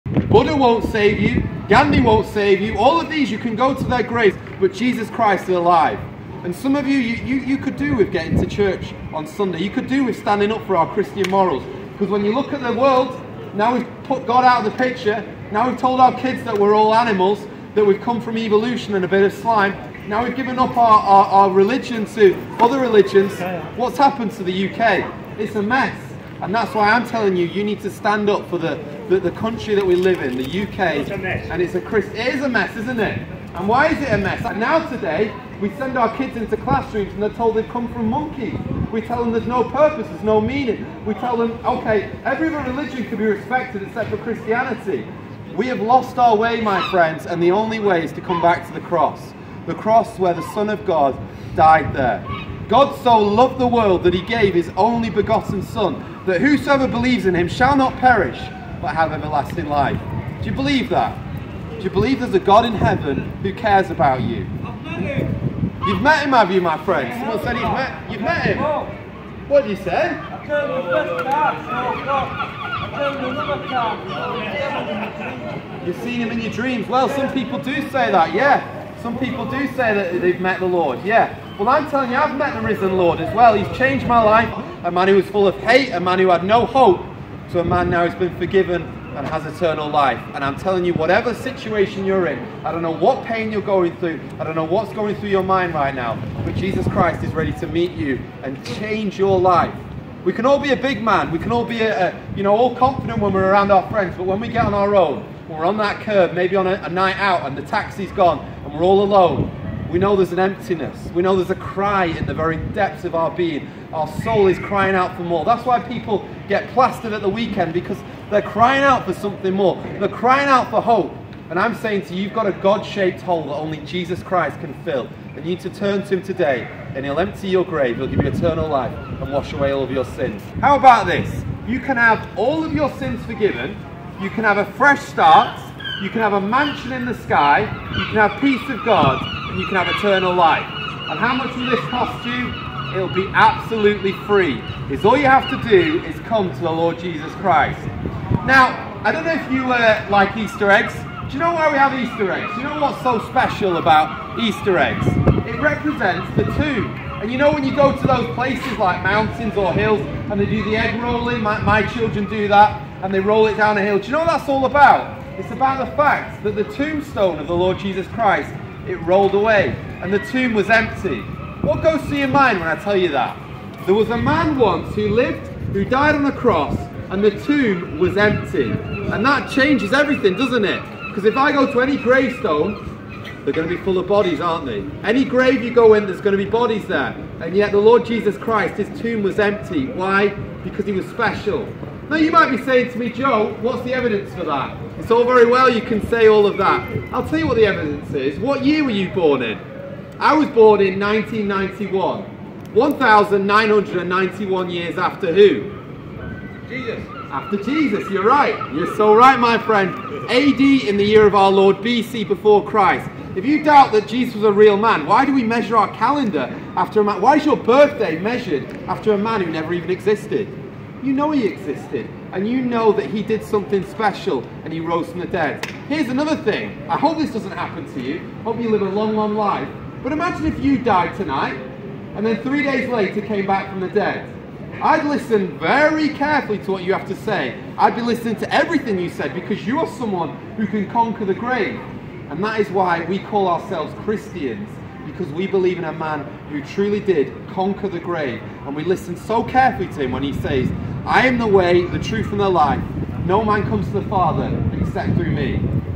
I Started Preaching Outside Wetherspoons in Morecambe Seaside Town (Interesting Reactions)
Not sure they had seen a street preacher before in this quiet seaside town, but praise God for those who heard the gospel!